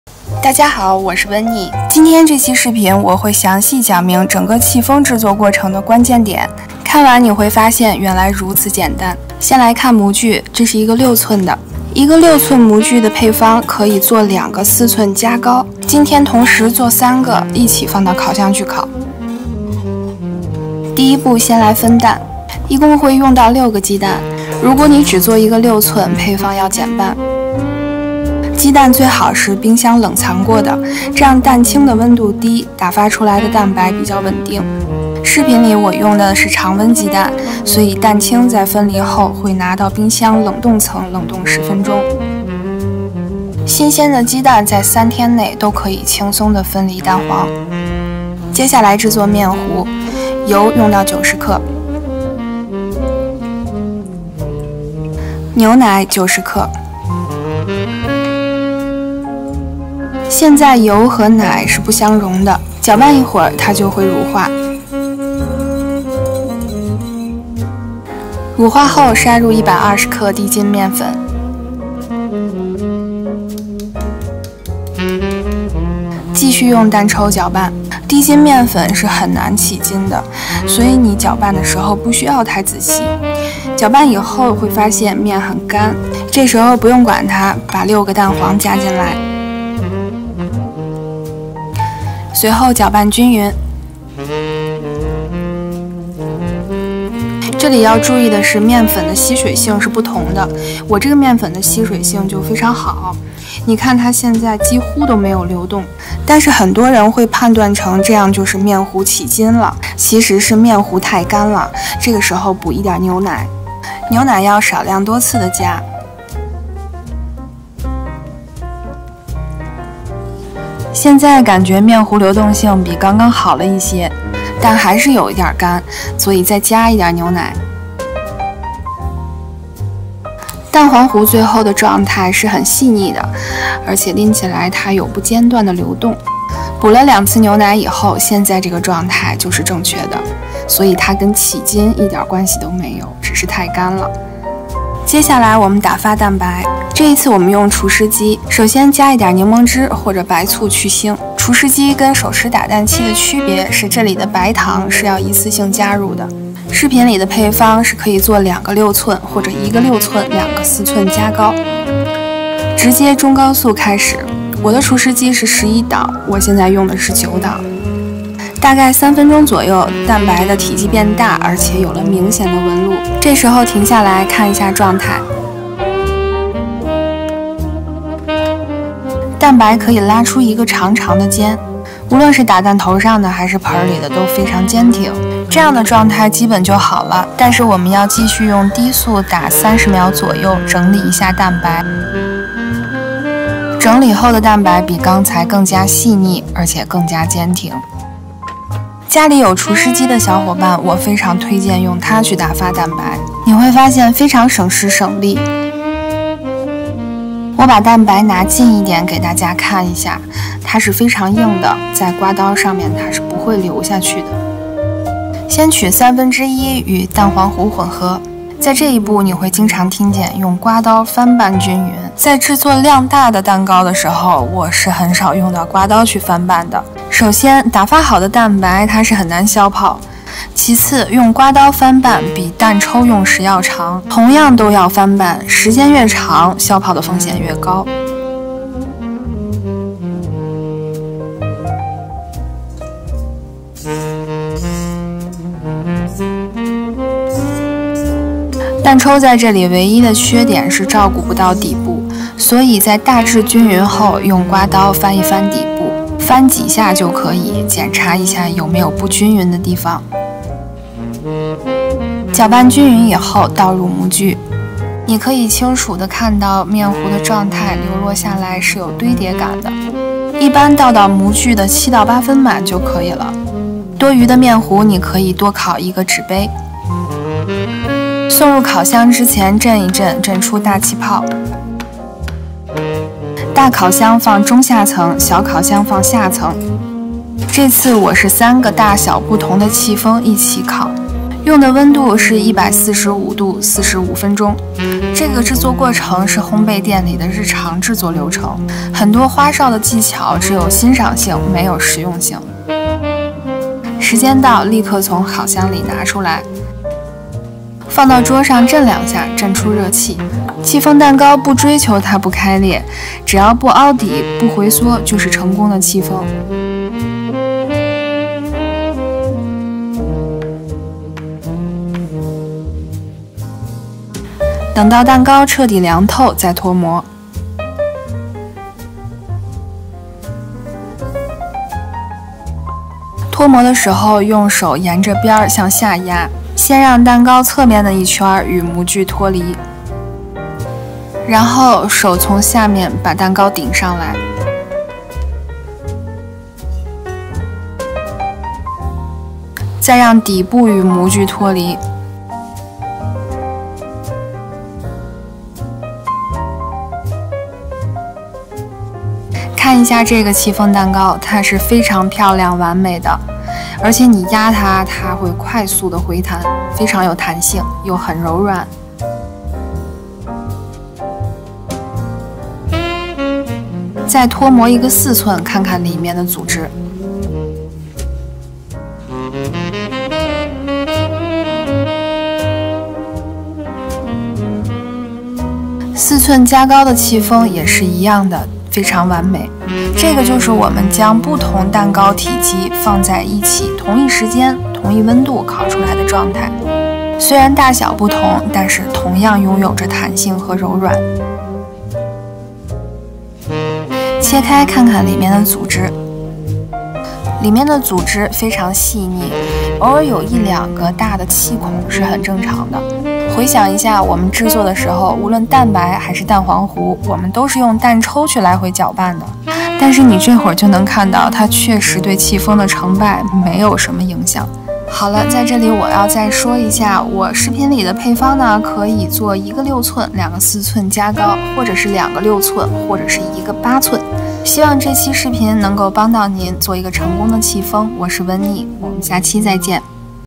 Chiffon easy follow戚風蛋糕 烘焙店日常制作流程 没有炫技 全部实用教程 一次就成功
拥有一家烘焙工作室，对于制作戚风蛋糕也有一些心得，视频的流程是店里日常的制作流程，希望我的分享能对您有所帮助。

戚风蛋糕
以下配方为一个6寸和两个四寸加高
只做一个6寸，配方请减半。
鸡蛋 6个
油 90g
牛奶 90g
低筋面粉 120g
白砂糖 100g
柠檬汁少许
145°C 45分钟，大烤箱中下层/小烤箱下层。

Owning a baking studio, I also have some experience in making chiffon cakes. The video process is the daily production process in the store. I hope my sharing can be helpful to you.

chiffon cake
The following recipe is a 6-inch and two 4-inch heightened
Only make one 6-inch, please halve the recipe.
6 eggs
Oil 90g
milk 90g
Low gluten flour 120g
White sugar 100g
a little lemon juice
145°C for 45 minutes, middle and lower shelf of large oven/lower shelf of small oven.